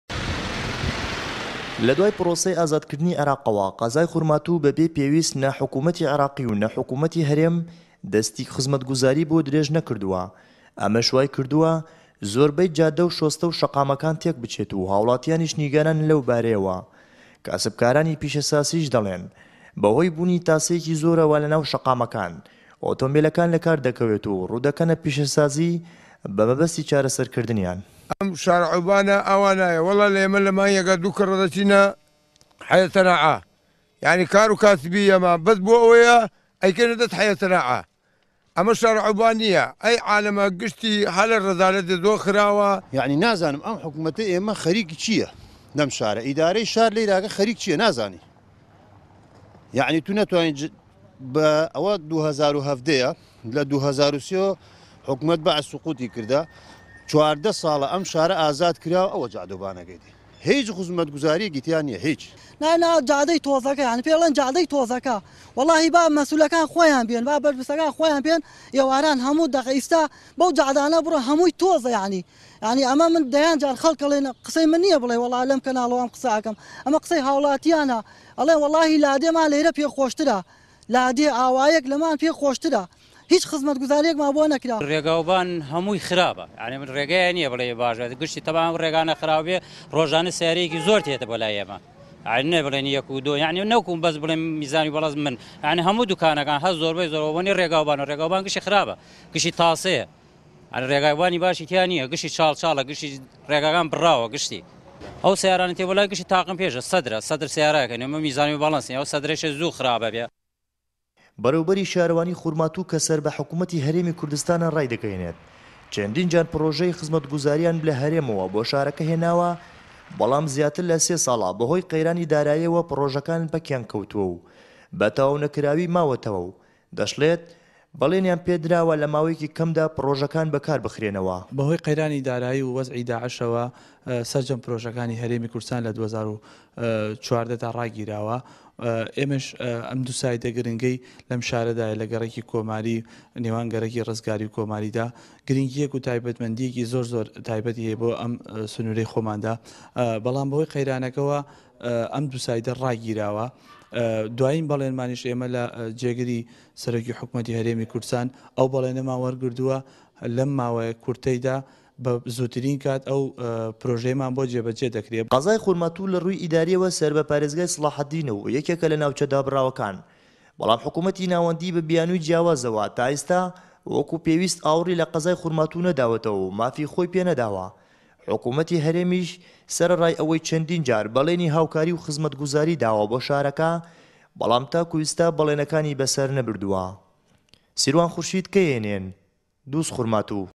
خورماتوو له‌ دوای پرۆسه‌ی ئازادی عێراقه‌وه‌ ئاوڕی لێ نه‌دراوه‌ته‌وه‌
له‌دوای پرۆسه‌ی ئازادی عێراقه‌وه‌  خورماتوو وه‌ك ناوچه‌یه‌كی دابڕاو له‌ هه‌رێمی كوردستان، ئاوڕی لێ نه‌دراوه‌ته‌وه‌ و زۆربه‌ی كوچه‌و كۆڵان ‌و شوێنه‌ گشتییه‌كانی به‌ره‌و وێرانبوون ده‌چن، به‌ڕێوه‌به‌ری شاره‌وانی قه‌زاكه‌ش ده‌ڵێت: حكومه‌تی هه‌رێم چه‌ند پرۆژه‌یه‌كی بۆ شاره‌كه‌ ته‌رخانكردووه‌، به‌ڵام به‌هۆی قه‌یرانی داراییه‌وه‌ جێبه‌جێ ناكرێن.